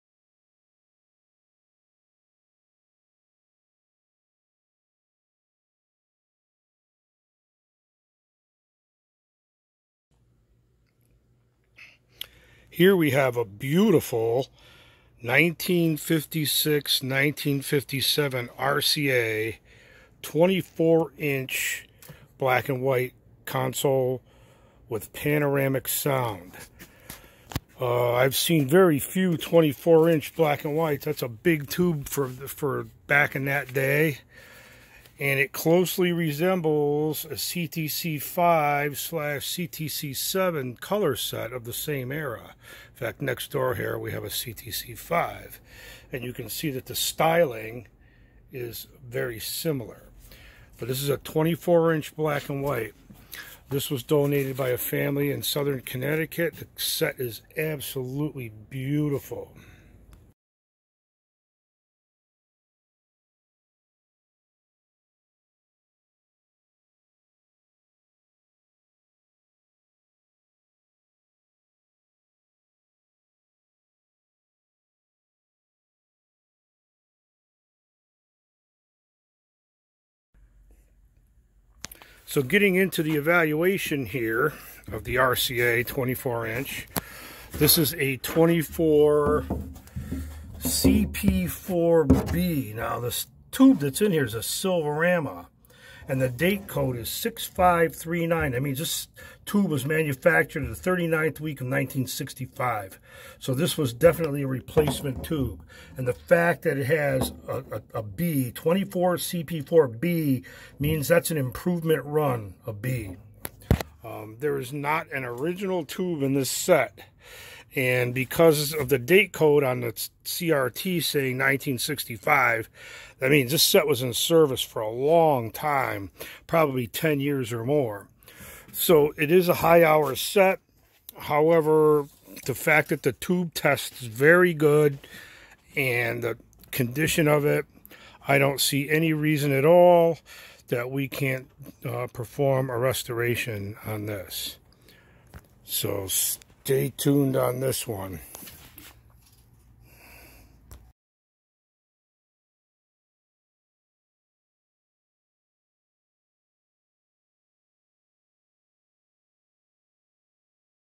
1956 RCA TV Evaluation
A beautiful RCA 24-D-7545 KCS 104AA TV cared for by the same family since new- We will do an electronic restoration.